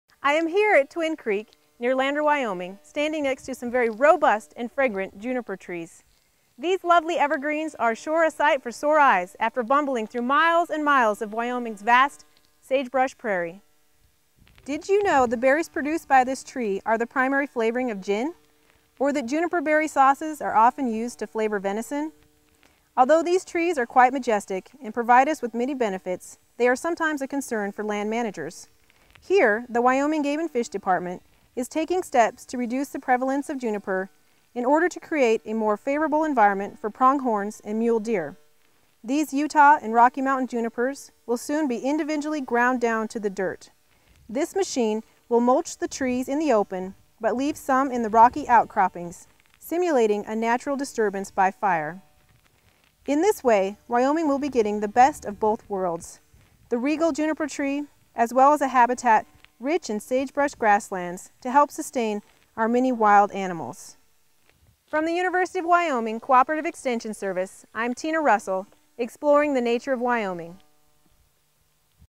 Juniper Control
Juniper is a plant which brings a mixed bag of benefits and problems, and the management challenges which accompany such a mix!